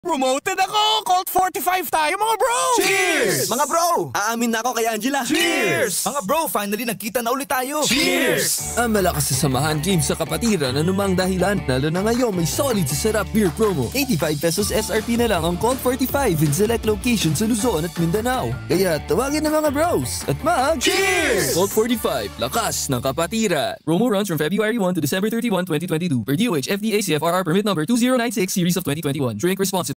Colt45 Solid Sa Sarap Beer Promo Radio Ad 2022 30s (Luzon and Mindanao)
This is the Radio Commercial for the Colt45 Solid Sa Sarap Beer Promo offering a price rollback on their Colt45 Products from February 1 to December 31, 2022. This promo is applicable to Luzon and Mindanao Stores only.

DRPS Two 1824: Colt45 Solid Sa Sarap Beer Promo Radio Ad 2022 30s (Luzon and Mindanao)